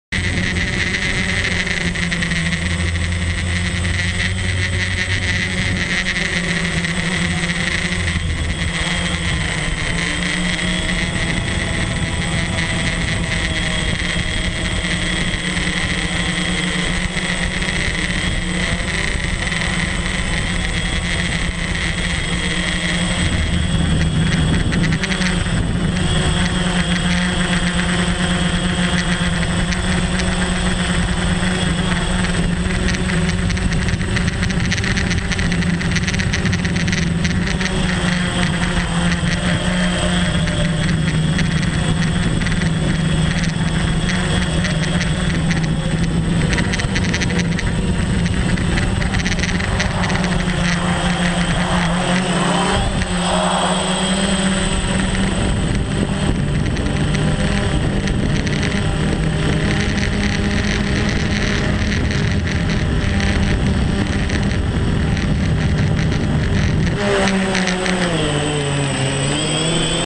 Bangkok Protests December 22 2013 - Aerial drone view at Asok Intersection
5:50 PM December 22 2013 Asok/Sukhumvit intersection

Filmed with a GoPro 3 on a DJI Phantom